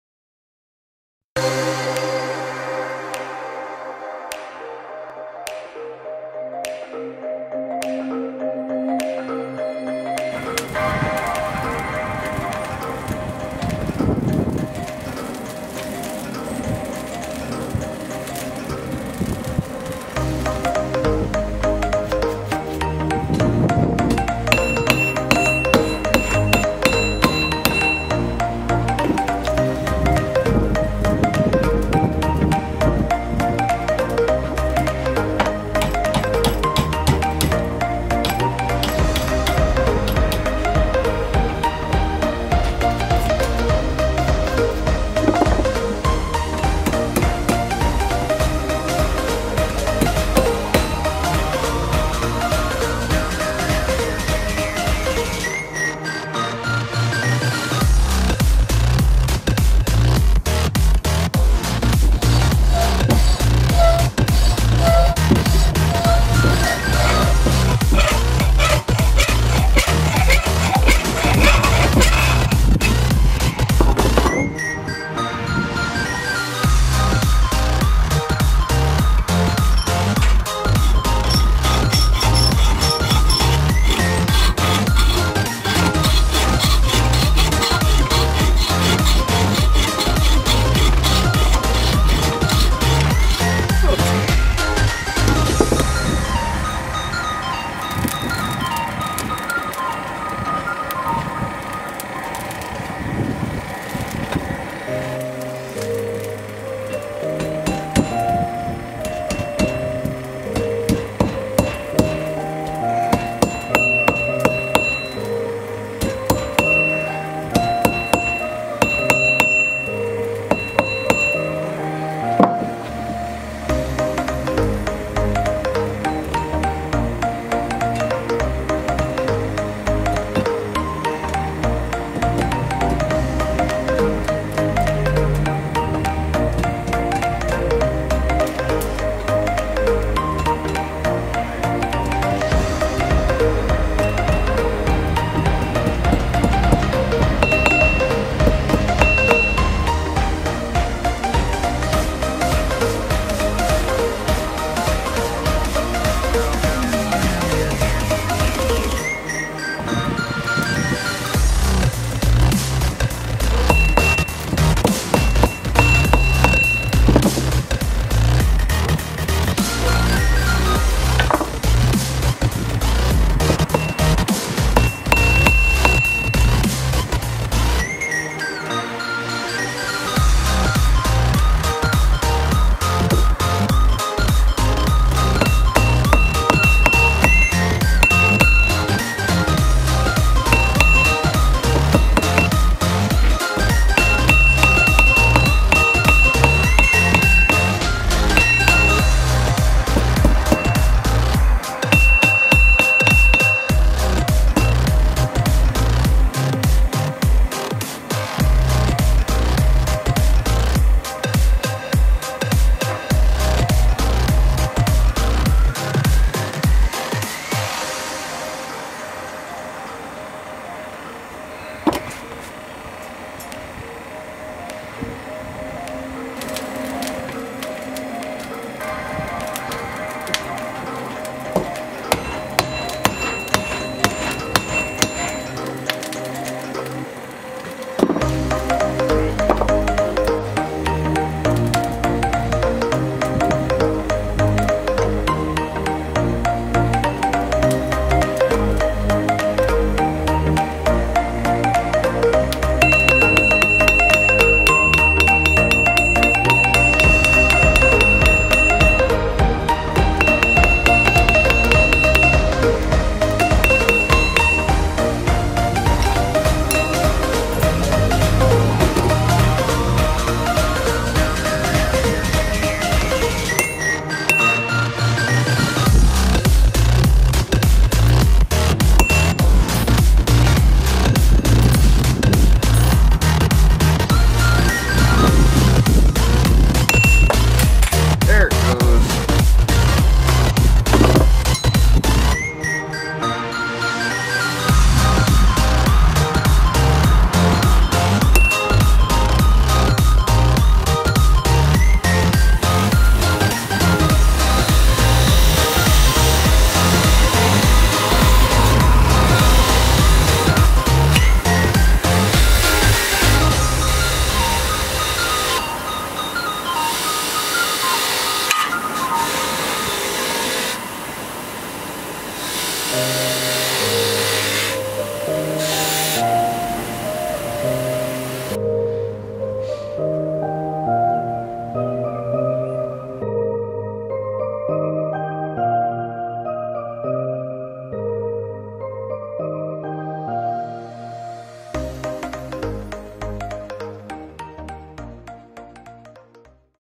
Forging a Steel Split Cross
How I forge a steel cross. I'm just learning how to smith and am probably doing lots wrong so don't judge to harshly.